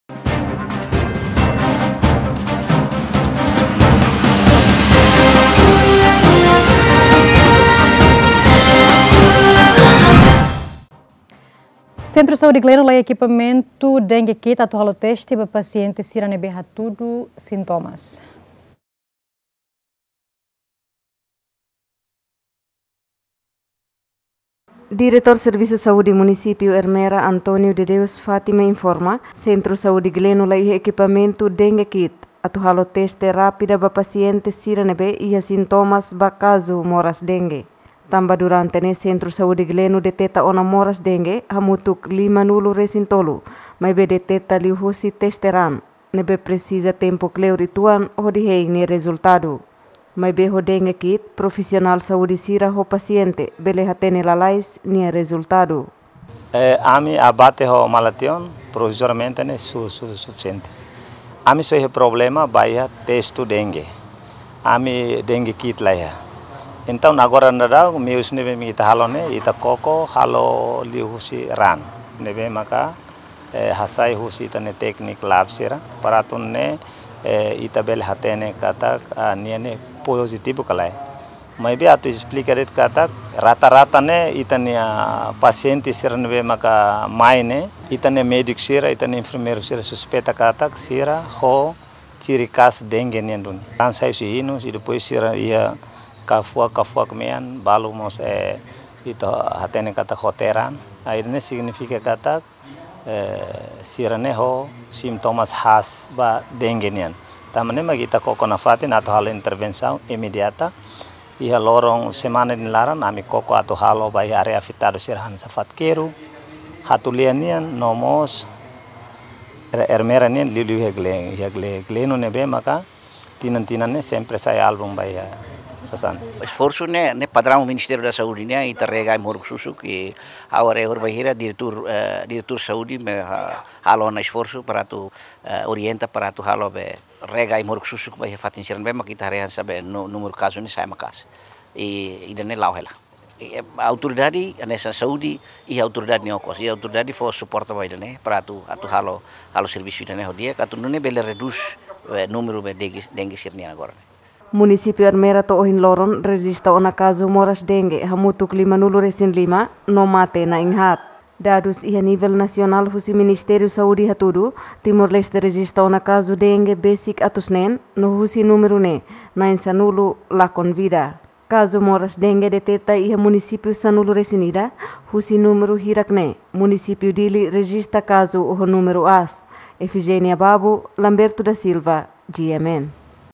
Sentru Saúde Gleno laiha ekipamentu Dengue-Kit teste pasiente
Sentru Saúde Glenu, Munisípiu Ermera, laiha ekipamentu Dengue-kit, atu halo teste rápida ba pasiente sira ne’ebé iha síntomas Dengue